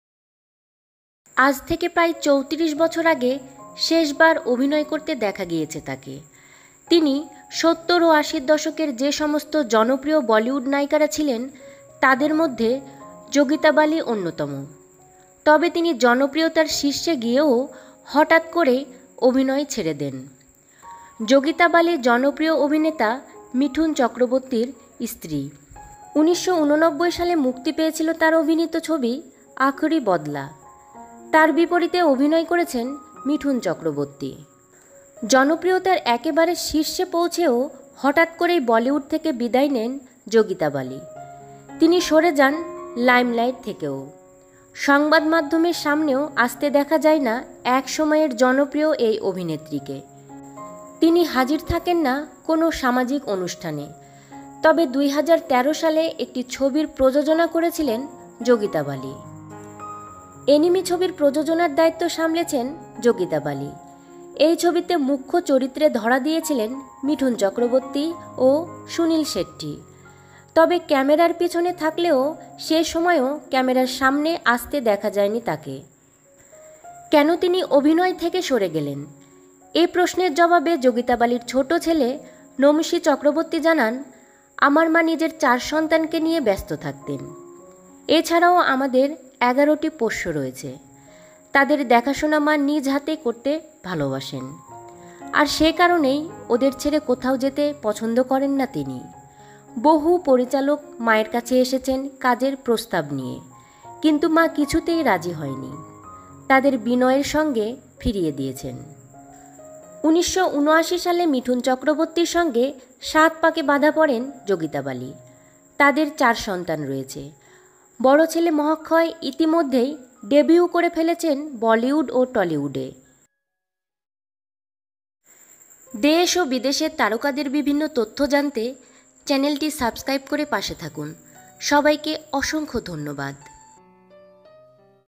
মিঠুন পত্নী যোগিতা বালি কোথায় কেমন আছেন? || Biography of yogita bali
Photo credit:- Source of internet/google/social media.

★Voice:- Own
★we have taken information collection from internet & reality show.